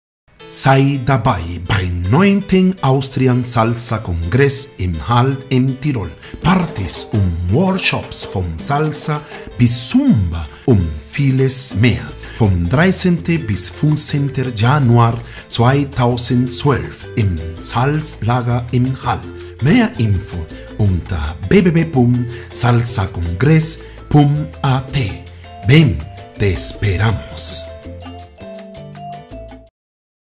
Salsa_Weekend_Festival_TVC_30_2011_H264_1080p.mov
Austrian Salsa Congress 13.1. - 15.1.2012
From Bachata to Salsa and ZUMBA Fitness - be part of the biggest latin event Austria